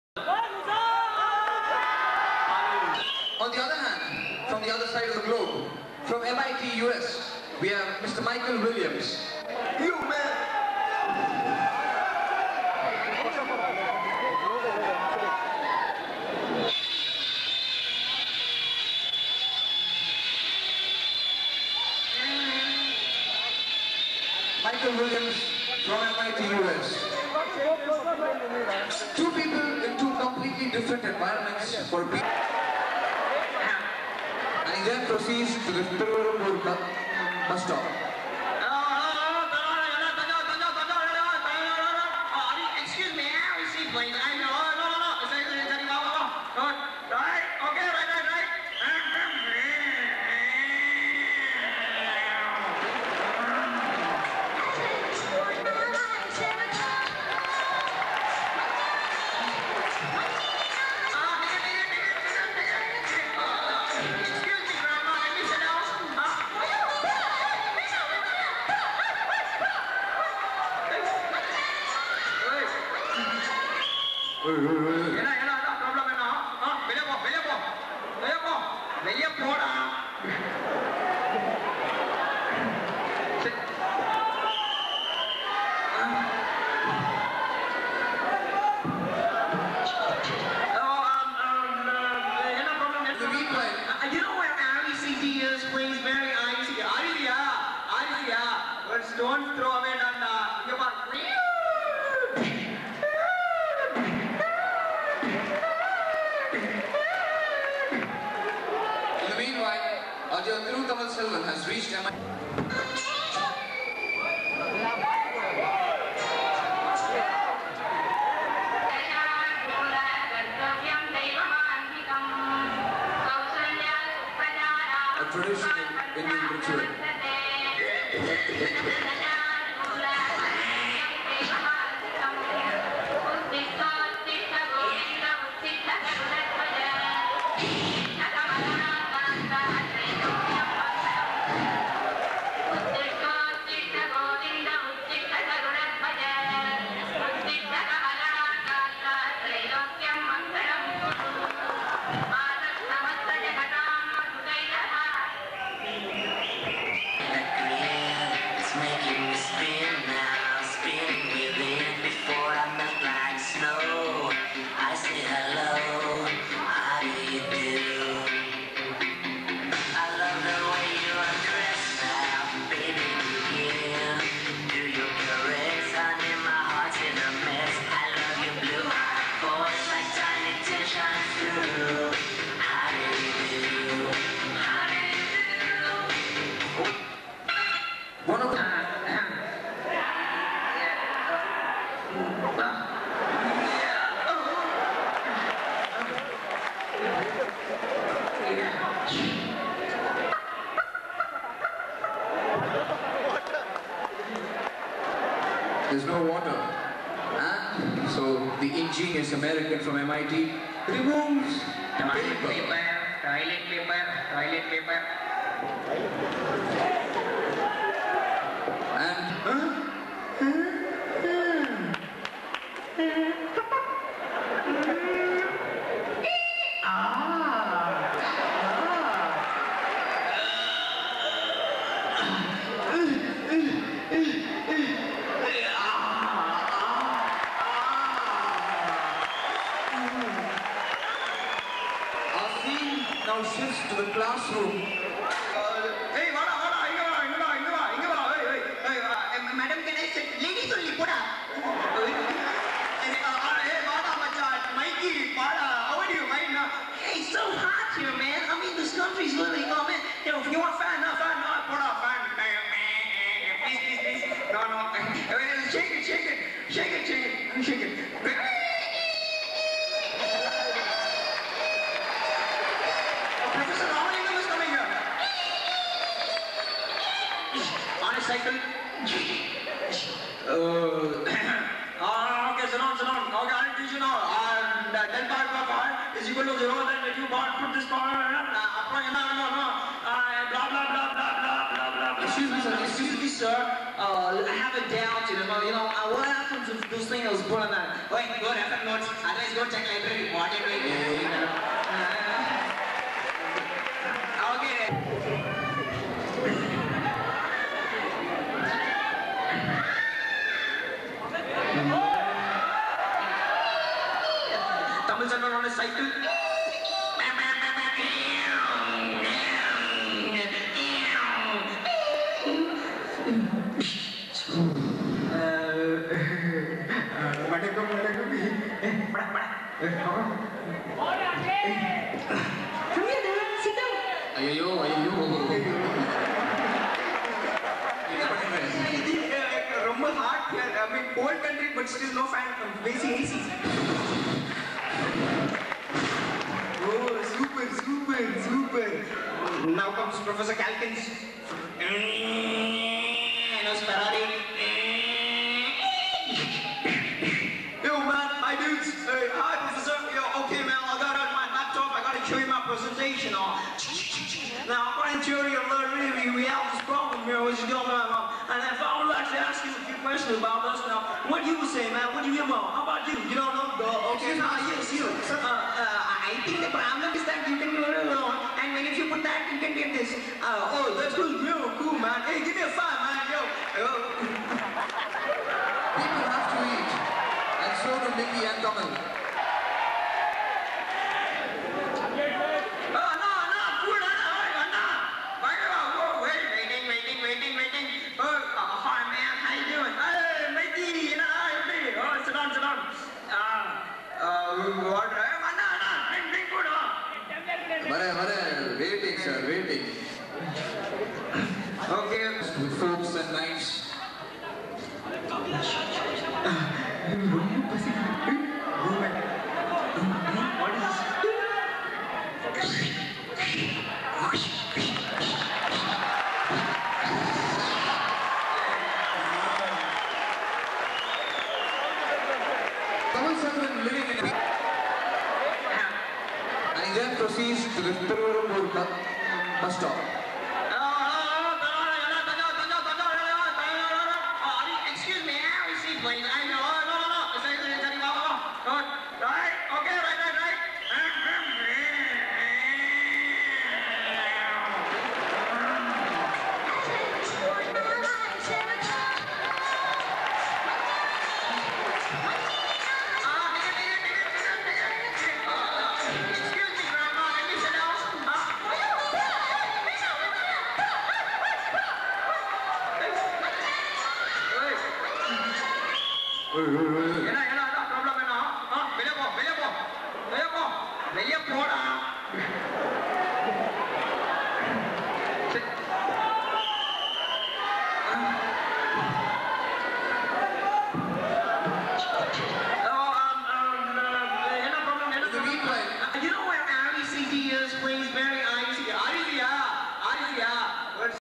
REC Trichy 1997 batch - CULFEST part 1
The MIT and RECT trade skit , back then it sounded very funny, now .. err, see for yourself!
The Pearl boys giving their last performance

Basha
Camera: Saudi